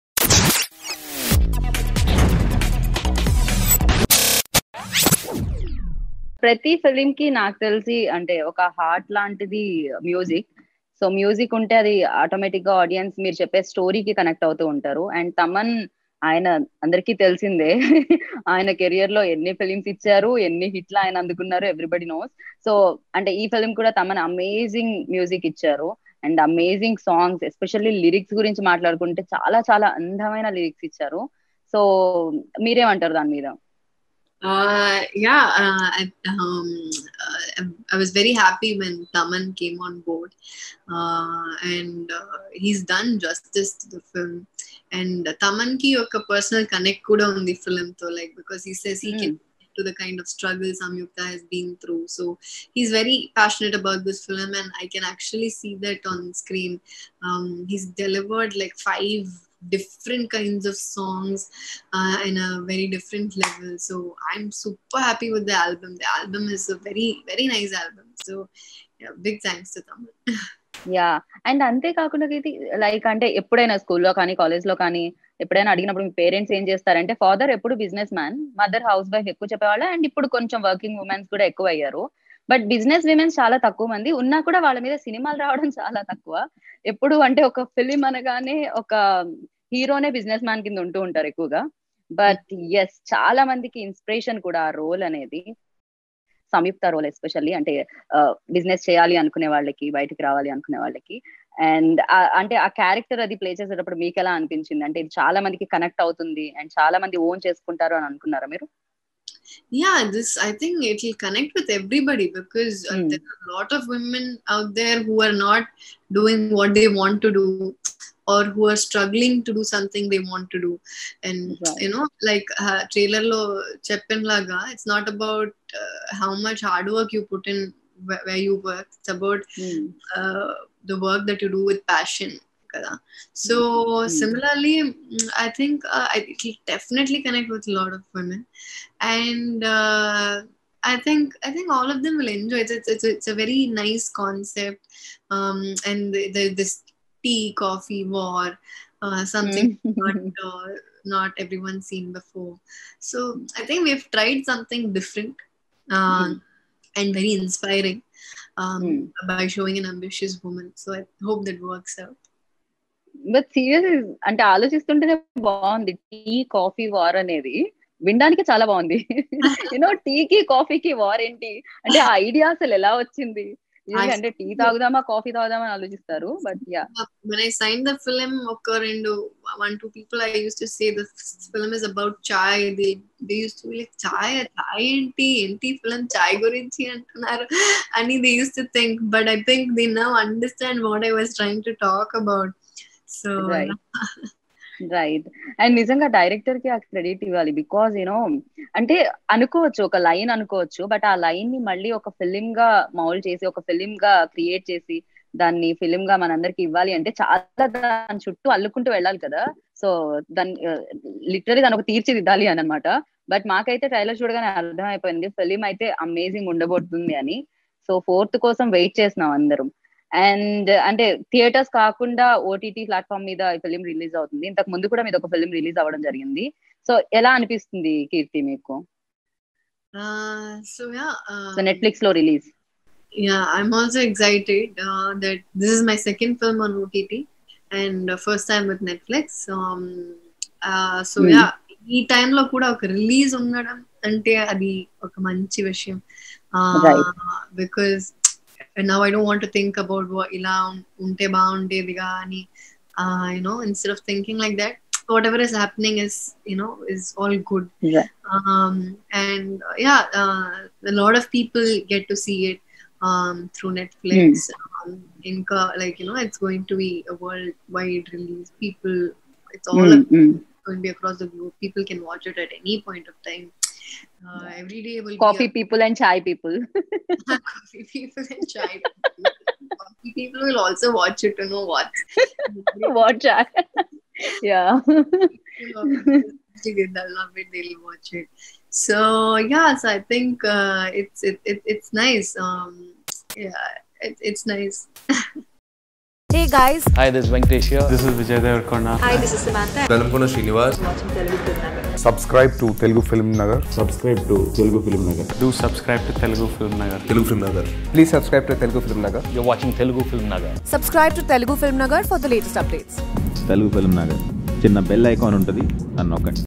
Keerthy Suresh about Thaman Music | Miss India Movie | Jagapathi Babu
Keerthy Suresh about Thaman Music on Telugu FilmNagar. Keerthy Suresh Exclusive Interview. MissIndia 2020 Latest Telugu Movie Ft Keerthy Suresh, Jagapathi Babu, Naveen Chandra, Rajendra Prasad, Naresh, Bhanushree Mehra, Sumanth S, Poojitha Ponnada, Kamal Kamaraju and Nadhiya. Directed By Narendra Nath. Music By Thaman. Produced By Mahesh S Koneru under East Coast Productions.